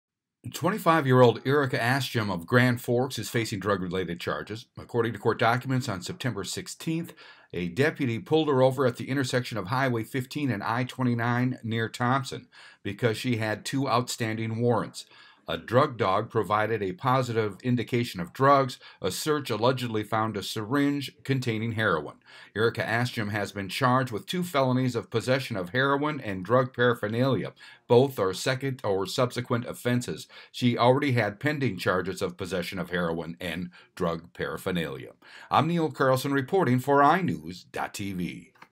Grand Forks Woman Facing More Drug Charges
GRAND FORKS, ND (iNewZ.TV) A Grand Forks woman is facing more drug charges, after being pulled over on a traffic stop for outstanding warrants.